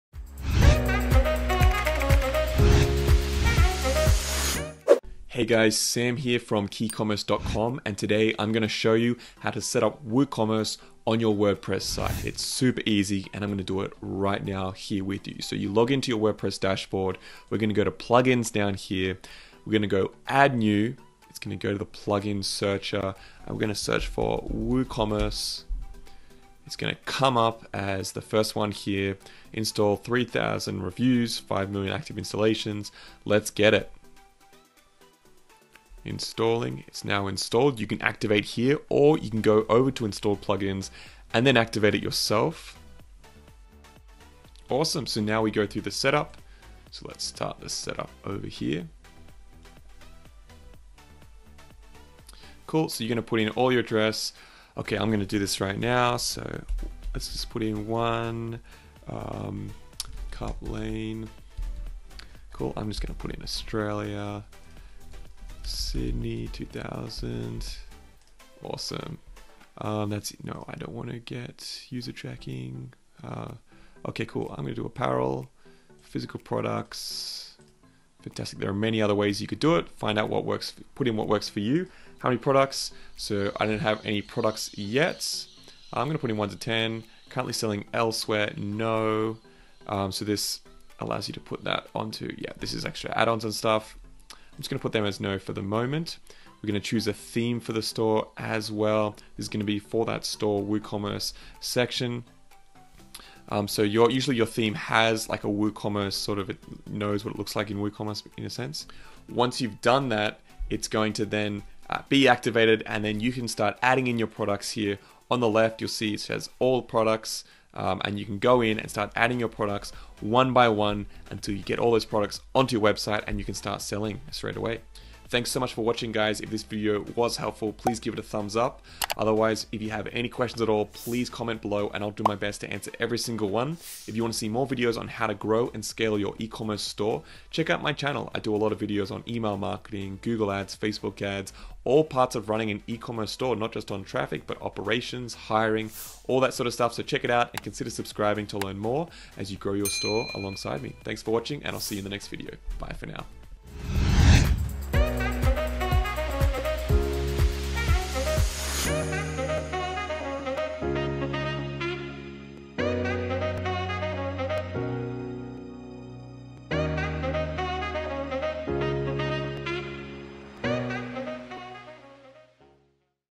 How to Install WooCommerce in Wordpress - Easy Full Setup Tutorial
How to Install WooCommerce in Wordpress? Just like any other plugin, installing WooCommerce is very easy! Follow along with me in this video to learn how to install and setup WooCommerce.

📅 Let us grow your Google Ads account for you. Book a free strategy session with Sam

In this video, I will take you through the full process of setting up the WooCommerce plugin on your WordPress site. I will show you how to install the plugin so you can start adding your products to your WooCommerce store.

If you’re planning to setup a WordPress-powered online store, you must consider installing WooCommerce. Thanks for its flexibility and wide variety of customization features, this plugin has become the top choice of Ecommerce merchants with WordPress sites. However, some WordPress users find installing WooComemrce a bit daunting because of the plethora of features it offers.

Fortunately, setting up WooCommerce is VERY EASY! Ecommerce newbies can easily download, install, and setup the plugin to have their online store up and running in no time. To install and setup WooCommerce in WorPress, follow these simple steps:
1. Go to: Plugins then click Add New.
2. In the search bar, type “WooCommerce”.
3. It will be the first plugin you will see in the search results. Click Install Now.
4. Click Activate Now.
5. Setup WooCommerce store.

An that's it! You have successfully installed and setup WooCommerce plugin. Easy right?

🕘 Timestamps:
0:00 Intro
0:15 Go to Plugins and search for WooCommerce
0:28 Install and Activate WooCommerce
0:45 Start Setting up your WooCommerce store
1:35 Choose a Theme
1:48 You can now start adding your products

SOCIAL